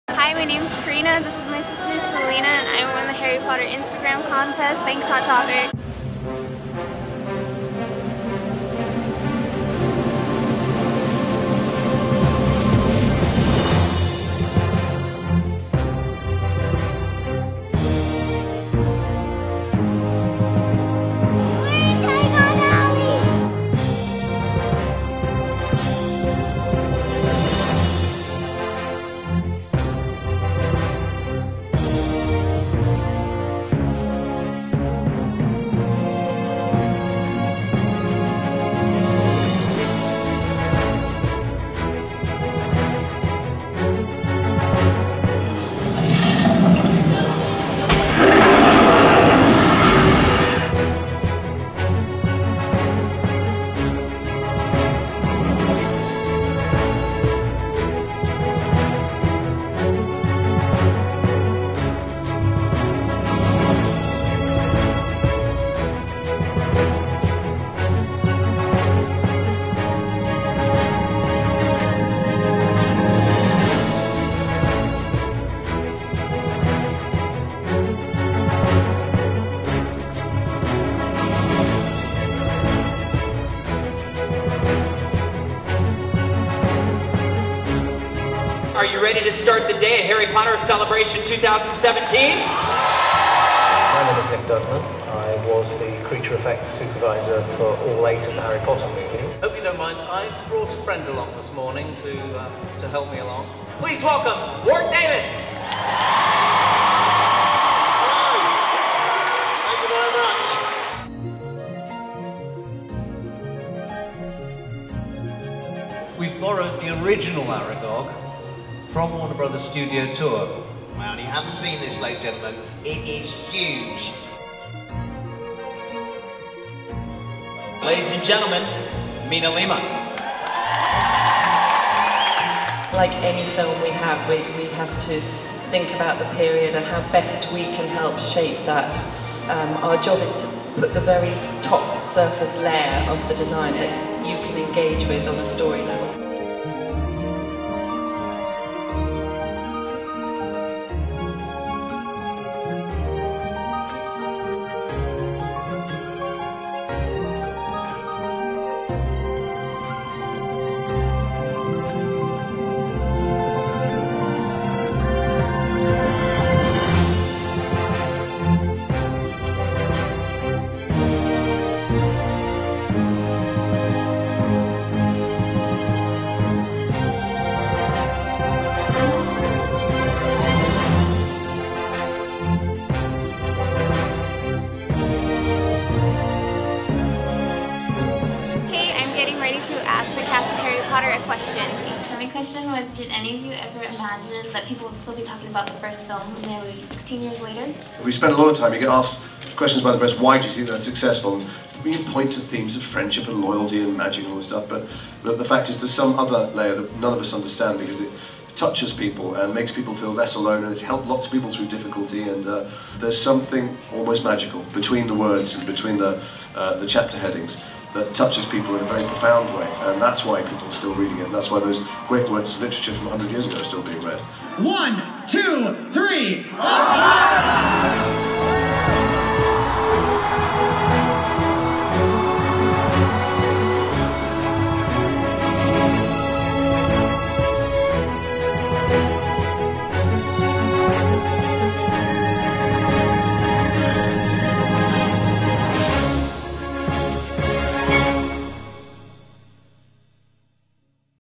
IWantToBeAWizard Contest Winner!
Corina entered our IWantToBeAWizard contest…and won! She even had the chance to ask Matthew Lewis the question she submitted in the contest. Watch the video and see all the exciting things she did at A Celebration of Harry Potter at Universal Orlando Resort.